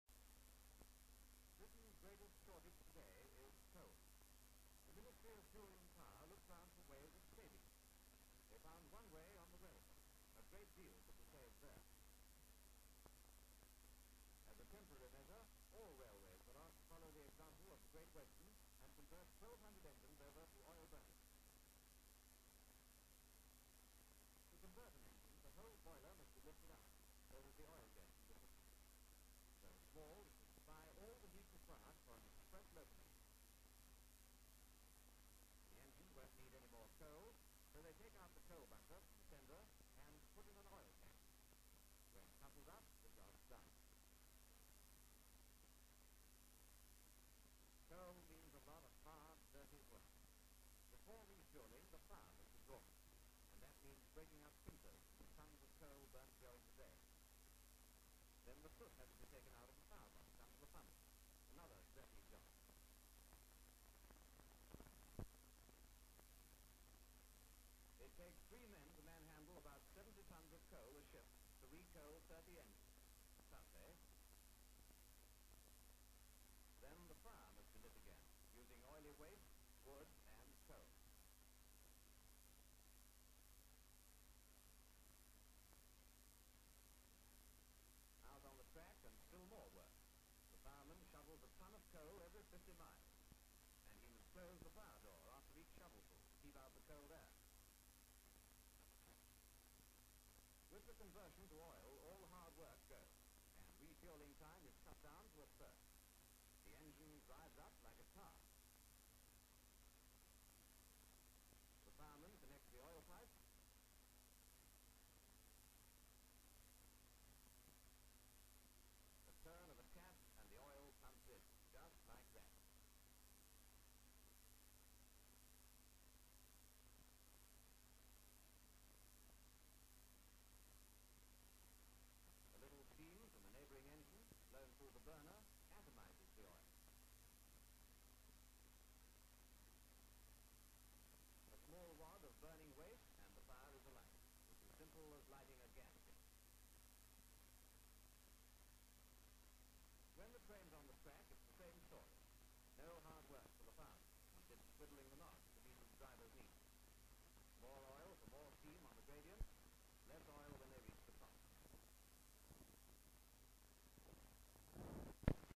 WW2 Locomotives, 1940's - Film 32540
Trains and railways in Britain during World War Two. Good shots of firing up boilers and so on. Oil fired GWR locos.  Locomotive 2862